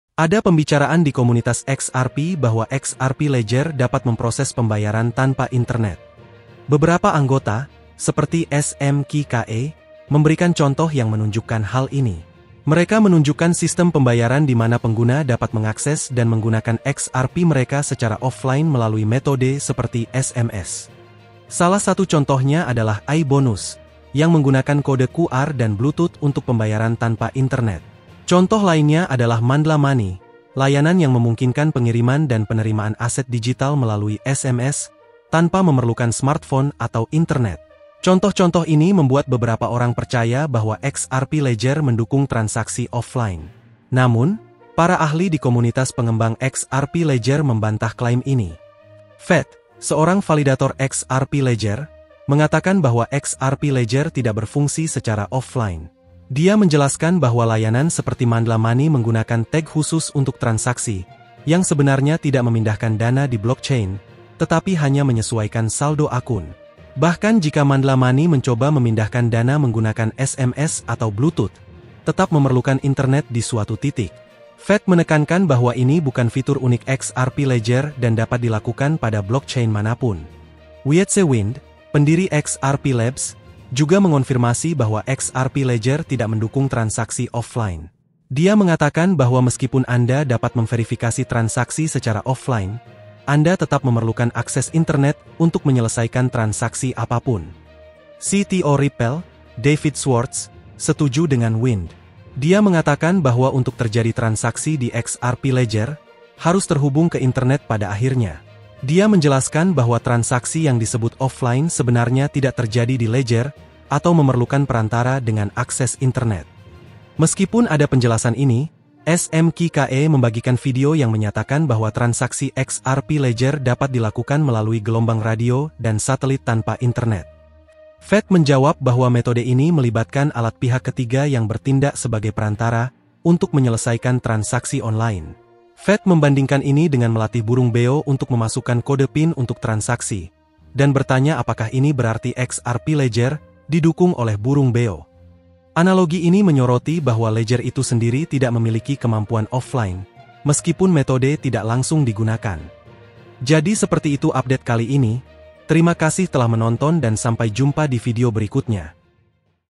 Bisakah Menggunakan XRP Ledger Tanpa Internet? CTO Ripple Beri Penjelasan di tengah Perdebatan!
Ada pembicaraan di komunitas XRP bahwa XRP Ledger (XRPL) dapat memproses pembayaran tanpa internet. Beberapa anggota, seperti "SMQKE," memberikan contoh yang menunjukkan hal ini. Mereka menunjukkan sistem pembayaran di mana pengguna dapat mengakses dan menggunakan XRP mereka secara offline melalui metode seperti SMS.

Salah satu contohnya adalah iBonus, yang menggunakan kode QR dan Bluetooth untuk pembayaran tanpa internet. Contoh lainnya adalah Mandla Money, layanan yang memungkinkan pengiriman dan penerimaan aset digital melalui SMS, tanpa memerlukan smartphone atau internet.

Contoh-contoh ini membuat beberapa orang percaya bahwa XRPL mendukung transaksi offline. Namun, para ahli di komunitas pengembang XRPL membantah klaim ini.

XRPL Tidak Mendukung Transaksi Offline
Vet, seorang validator XRPL, mengatakan bahwa XRPL tidak berfungsi secara offline. Dia menjelaskan bahwa layanan seperti Mandla Money menggunakan tag khusus untuk transaksi, yang sebenarnya tidak memindahkan dana di blockchain, tetapi hanya menyesuaikan saldo akun.

Bahkan jika Mandla Money mencoba memindahkan dana menggunakan SMS atau Bluetooth, tetap memerlukan internet di suatu titik. Vet menekankan bahwa ini bukan fitur unik XRPL dan dapat dilakukan pada blockchain manapun.

Wietse Wind, pendiri XRPL Labs, juga mengonfirmasi bahwa XRPL tidak mendukung transaksi offline. Dia mengatakan bahwa meskipun Anda dapat memverifikasi transaksi secara offline, Anda tetap memerlukan akses internet untuk menyelesaikan transaksi apa pun.

Kesepakatan CTO Ripple
CTO Ripple, David Schwartz, setuju dengan Wind. Dia mengatakan bahwa untuk terjadi transaksi di XRPL, harus terhubung ke internet pada akhirnya. Dia menjelaskan bahwa transaksi yang disebut offline sebenarnya tidak terjadi di ledger atau memerlukan perantara dengan akses internet.

Meskipun ada penjelasan ini, SMQKE membagikan video yang menyatakan bahwa transaksi XRPL dapat dilakukan melalui gelombang radio dan satelit tanpa internet. Vet menjawab bahwa metode ini melibatkan alat pihak ketiga yang bertindak sebagai perantara untuk menyelesaikan transaksi online.

Vet membandingkan ini dengan melatih burung beo untuk memasukkan kode PIN untuk transaksi, dan bertanya apakah ini berarti XRPL "didukung oleh burung beo." Analogi ini menyoroti bahwa ledger itu sendiri tidak memiliki kemampuan offline, meskipun metode tidak langsung digunakan.